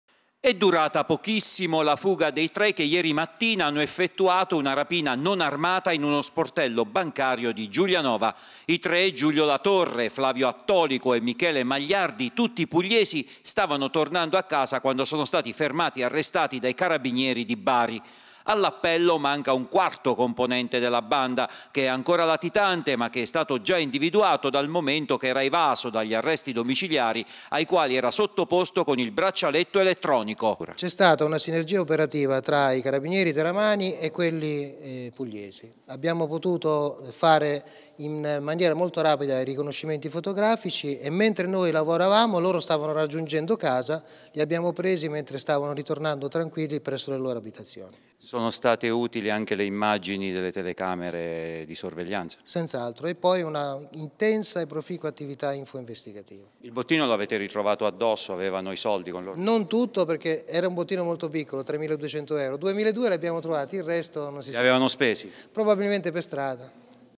Arrestati i tre rapinatori delle Popolare di Giulianova
Sono stati arrestati i rapinatori che ieri hanno messo a segno un colpo in banca a Giulianova. In meno di 6 ore i carabinieri di Teramo e Giulianova sono arrivati a scovare i rapinatori fino in Puglia, grazie alla collaborazione dei carabinieri di Bari.